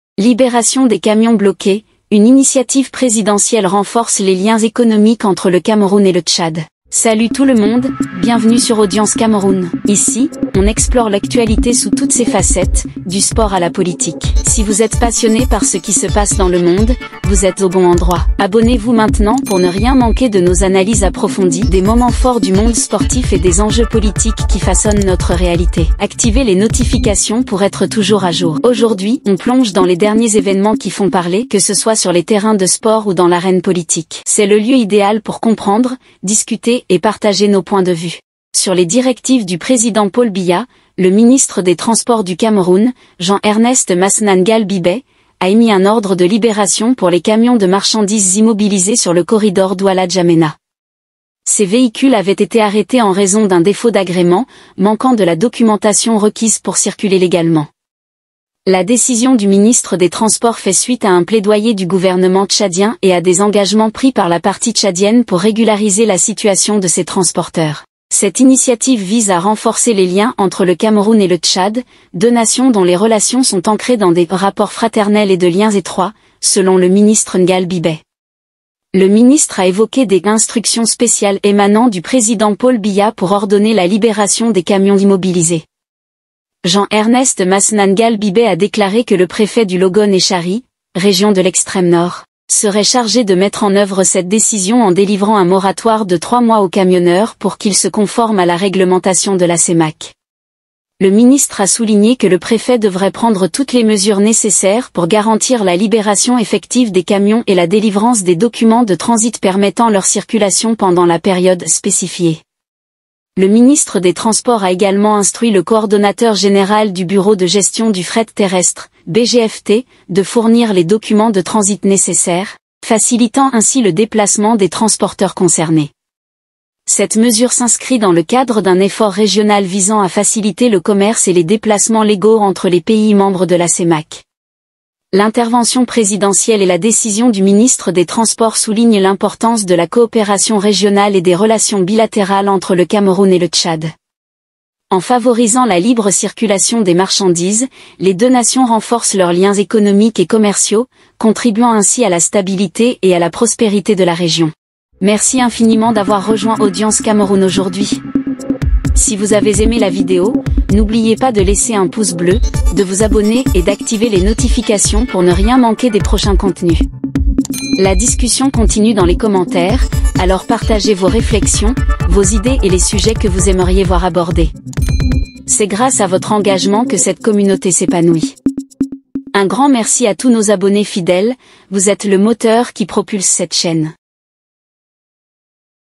Libération des camions bloqués : Une Initiative qui Renforce les Liens entre le Cameroun et le Tchad
Cette décision présidentielle du Cameroun de libérer les camions bloqués sur le corridor Douala-Ndjamena témoigne d'un engagement fort en faveur de la coopération régionale et renforce les liens économiques avec le Tchad#Cameroun